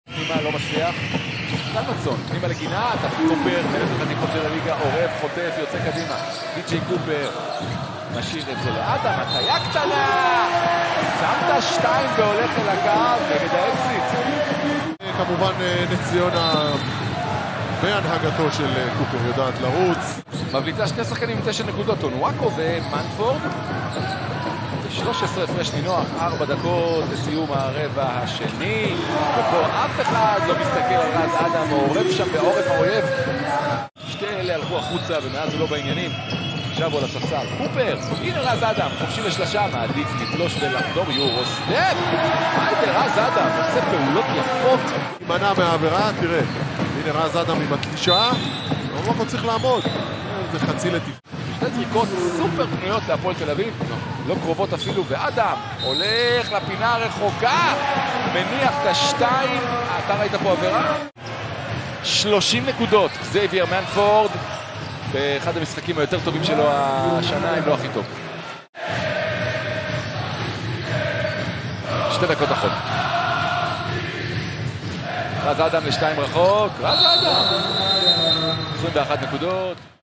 Raz Adam Points in Ironi Hai Motors Ness Ziona vs. Hapoel Vegan Friendly Tel Aviv
Raz Adam Points in Ironi Hai Motors Ness Ziona vs. Hapoel Vegan Friendly Tel Aviv, 05/20/2023